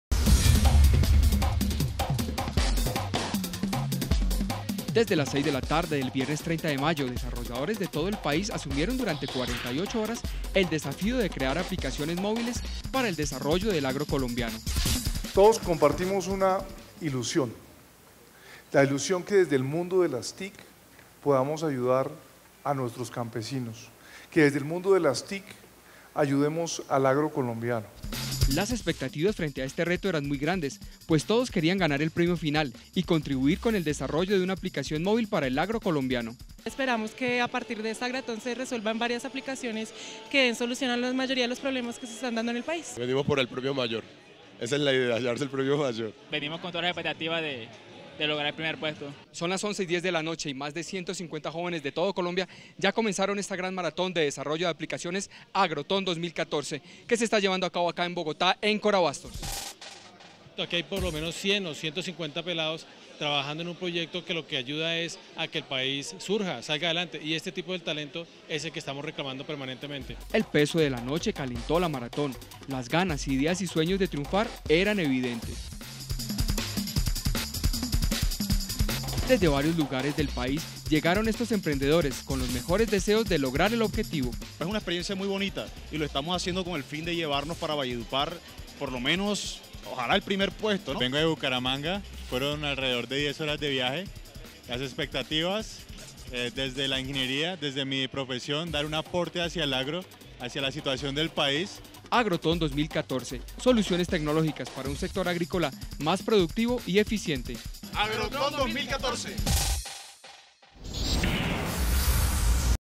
¿Qué es Agroton?
En Colombia por primera vez  se realizará una gran maratón de desarrollo de aplicaciones para el agro colombiano. Jóvenes rurales, desarrolladores, entidades públicas y empresas privadas se unen por una sola causa: soluciones tecnológicas para un sector agrícola más eficiente, productivo y sostenible.